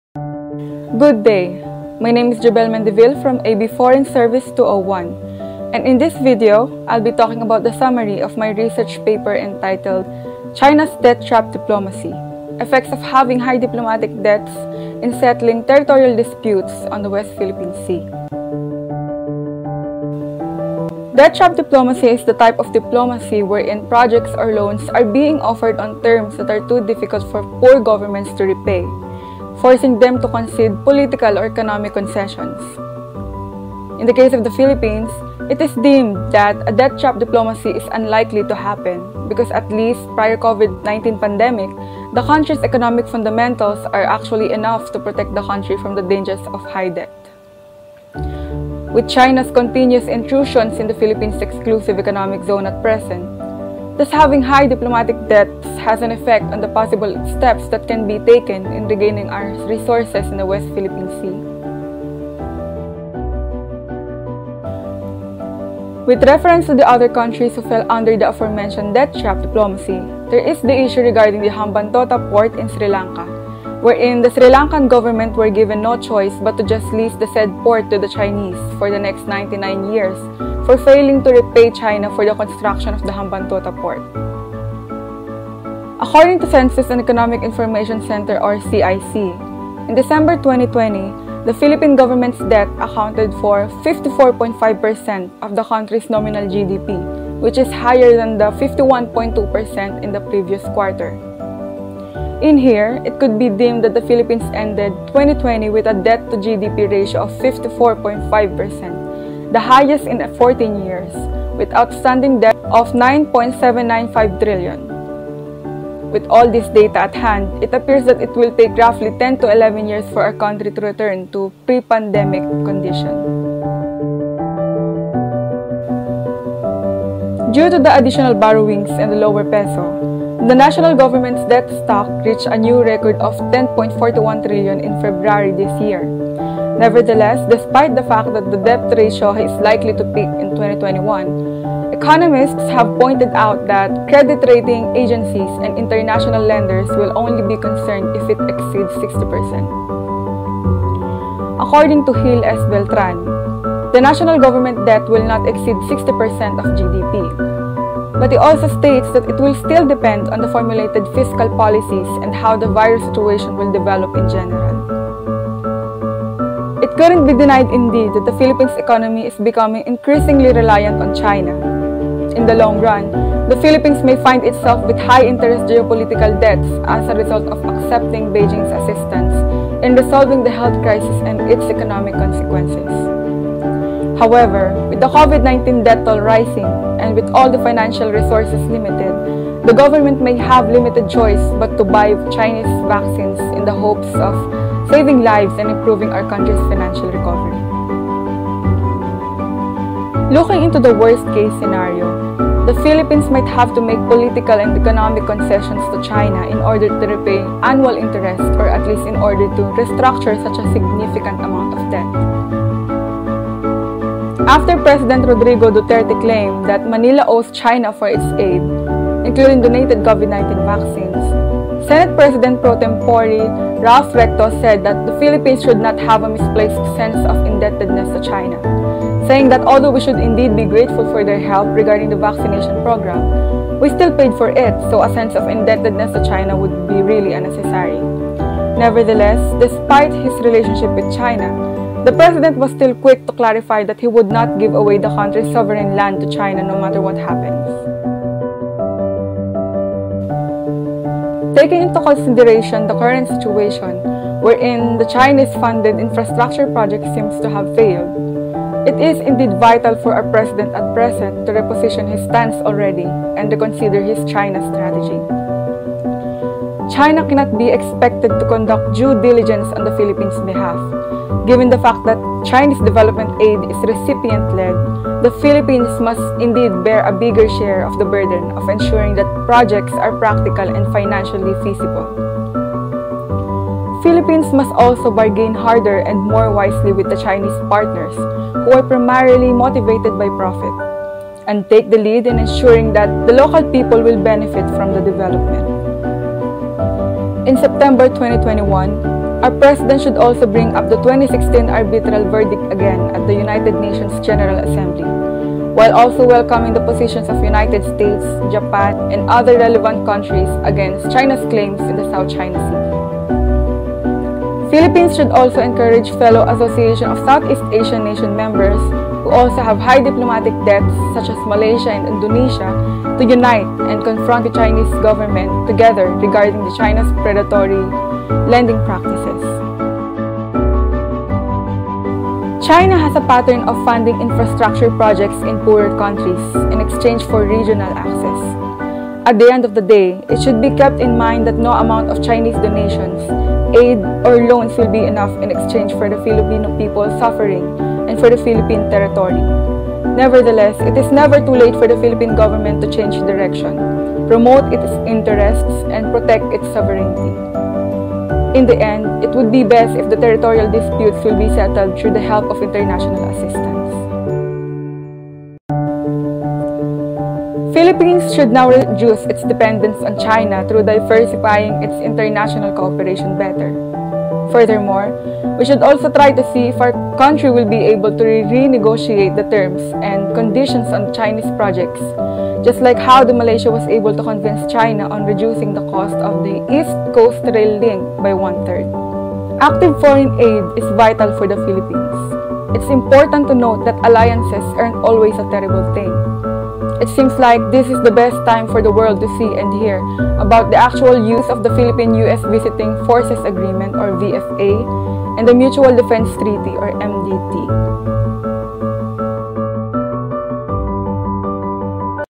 Effects of Having High Diplomatic Debts in Settling Territorial Disputes on West Philippine Sea
Name: Mendevil, Joebhel L.
Course & Section: AB Foreign Service 201
Subject: Political and Economic Geography (PEGN03F)
Professor: Mr. Jumel G. Estrañero

This is a video summary of my research paper entitled: “China’s Debt Trap Diplomacy: Effects of Having High Diplomatic Debts in Settling Territorial Disputes on West Philippine Sea”, made as a requirement for our final term this second semester.